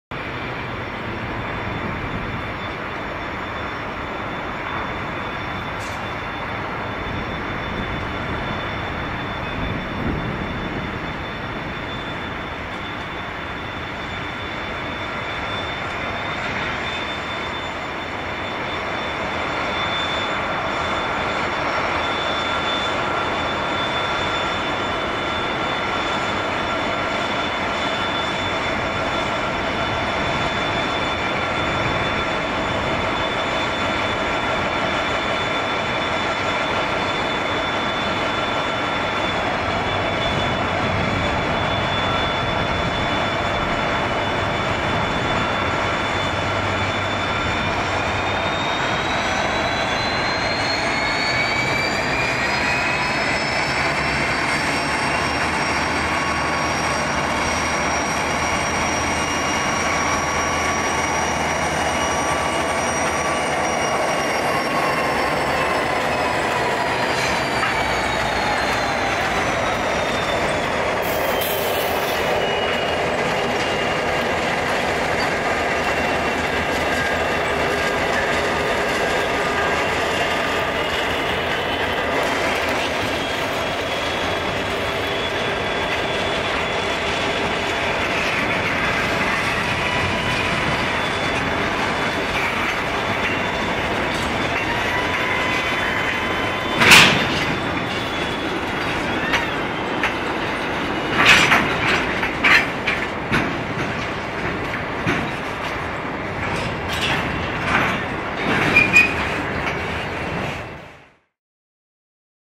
QRNational Contain Freight Train With LDP002, LDP007 & 42105 (24/9/2009) - PoathTV Railways
A QRNational (QRN, now Aurizon) freight train destined for Sydney prepares to depart Dynon (Melbourne) freight yards. The train has two LDP class diesel locomotives, Interail locomotive 42105, and container wagons.

Locoation: North Dynon (Melbourne), 24/9/2009
Locomotives: LDP002 + LDP007 + 42105

PoathTV